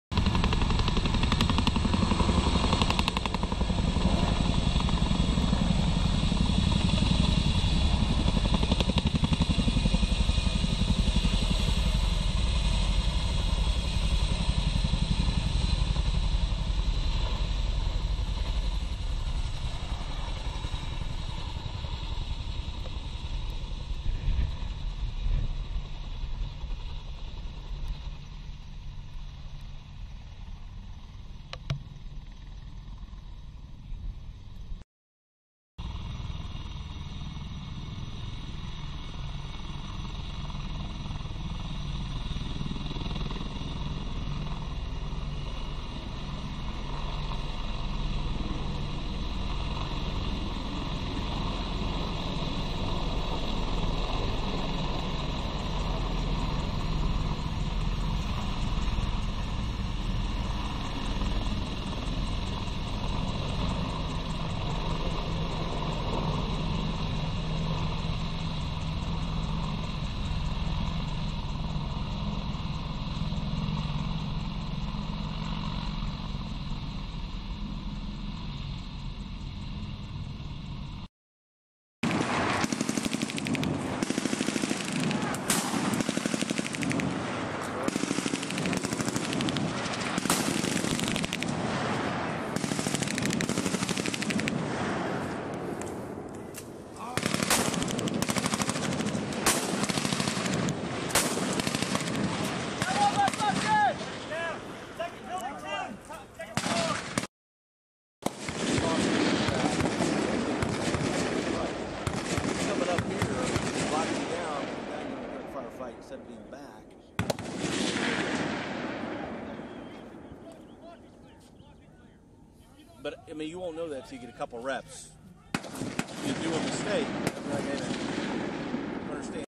76th IBCT at JRTC B-Roll
Credit: Master Sgt. Brad Staggs | Date Taken: 08/01/2017
The Indiana National Guard's 76th Infantry Brigade Combat Team is training at the Joint Readiness Training Center at Fort Polk, Louisiana. The following B-Roll was captured during their rotation in "the box".1. (0:00:00 - 0:34:20) Two Georgia National Guard CH-47 Chinooks take off from FOB Forge.2. (0:35:20 - 01:20:26) Two North Carolina National Guard AH-64 Apache Helicopters take off from FOB Forge.3. (01:21:26 - 01:47:01) Indiana National Guard Soldiers with Bravo Company, 1st Battalion, 293rd Infantry Regiment, 76th Infantry Brigade Combat Team react to fire at the Peason Ridge Live Fire Range at the Joint Readiness Training Center.4. (01:48:01 - 02:09:06) Indiana National Guard Soldiers with Bravo Company, 1st Battalion, 293rd Infantry Regiment, 76th Infantry Brigade Combat Team attack the enemy at the Peason Ridge Live Fire Range at the Joint Readiness Training Center.